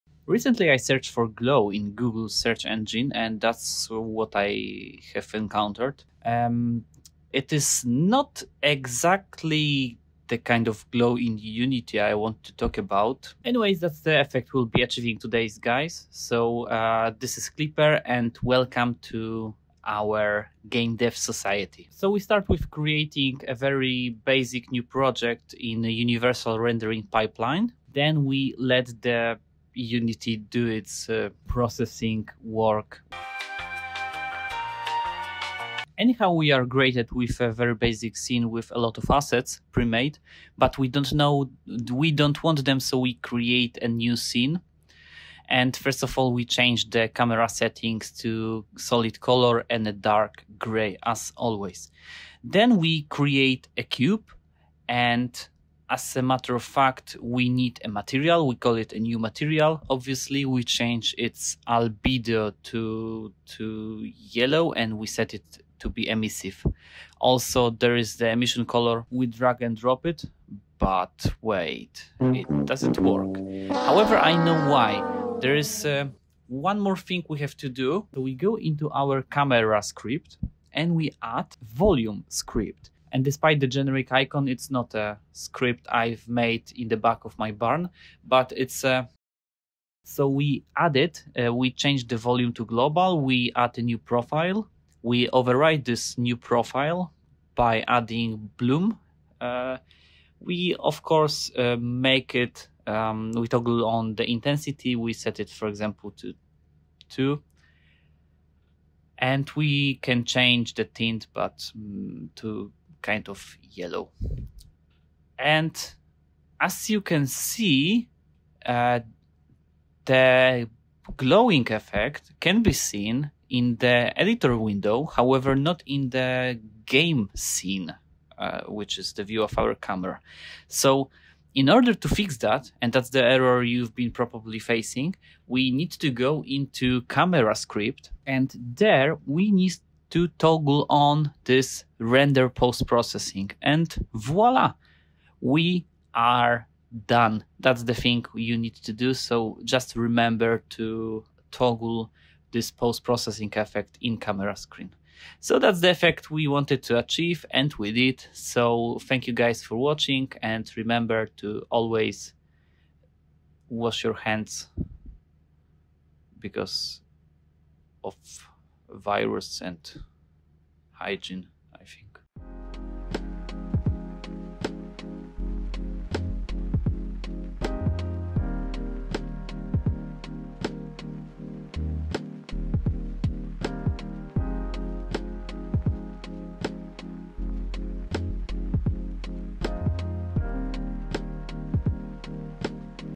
THE SIMPLEST GLOW EFFECT IN UNITY URP - TUTORIAL
Want to talk video games? Come and join GAME DEVils od Discord

Well, it's been several years... that notwithstaning I emerge once again with a tutorial that you have wanted so deeply. Today I show you how to achieve glow effect in Universal Rendering Pipeline using bloom in Unity 2019.3 and I suppose later.